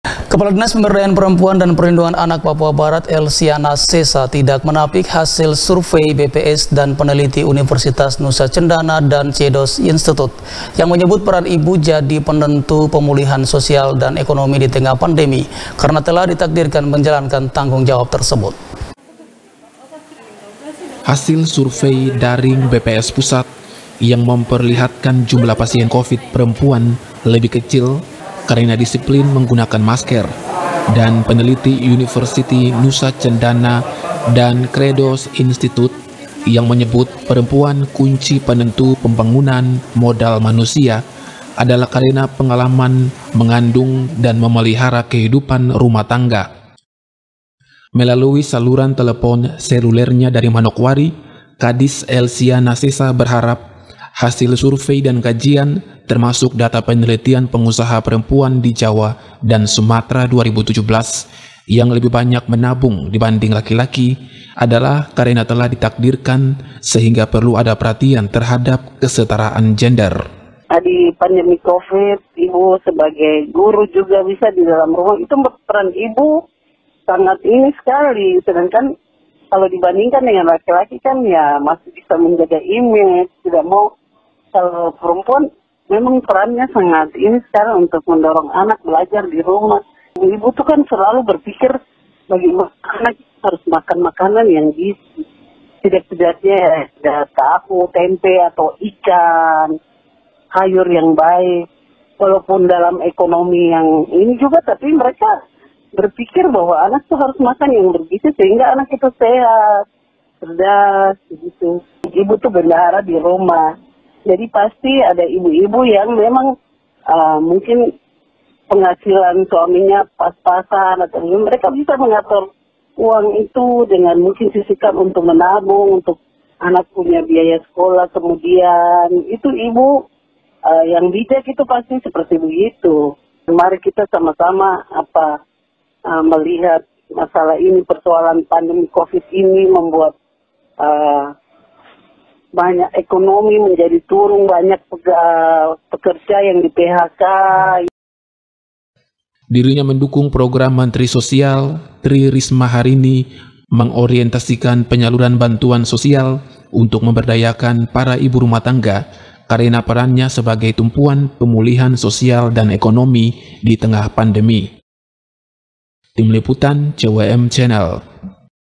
PERAN IBU PULIHKAN SOSIAL EKONOMI KELUARGA
KADIS PEMBERDAYAAN PEREMPUAN DAN PERLINDUNGAN ANAK PAPUA BARAT ELSIANA SESA TIDAK MENAMPIK HASIL SURVEI BPS DAN PENELITI UNIVERSITAS NUSA CENDANA DAN CREDOS INSTITUTE YANG MENYEBUT PERAN IBU JADI PENENTU PEMULIHAN SOSIAL DAN EKONOMI DITENGAH PANDEMI KARENA TELAH DITAKDIRKAN MENJALANKAN TANGGUNG JAWAB TERSEBUT.